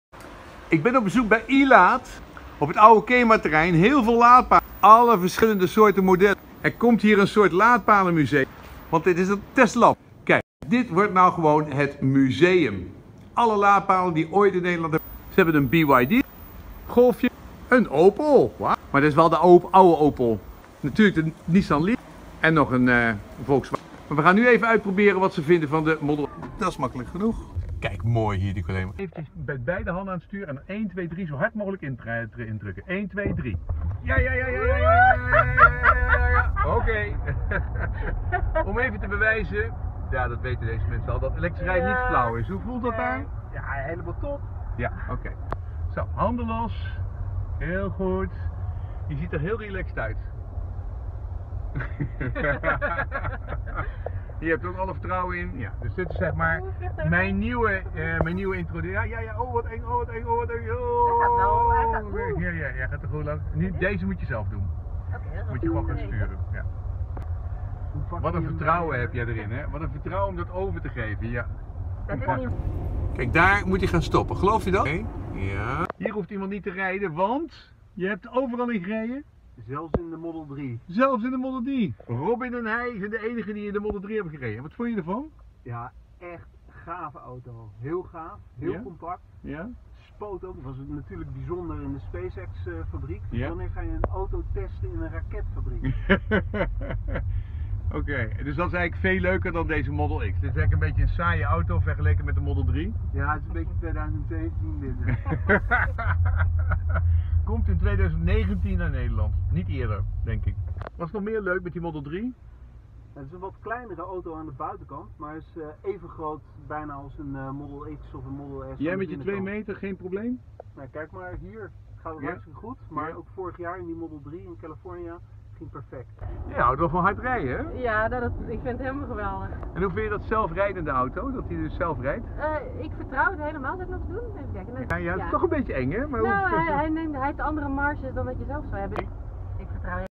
Met @driessenlonneke en @BaertedeBrey van @elaadNL in de modelx @TeslaMotors EV/autopilot Testen
Met @driessenlonneke en @BaertedeBrey van @elaadNL in de modelx @TeslaMotors even EV en zelfrijdende auto testen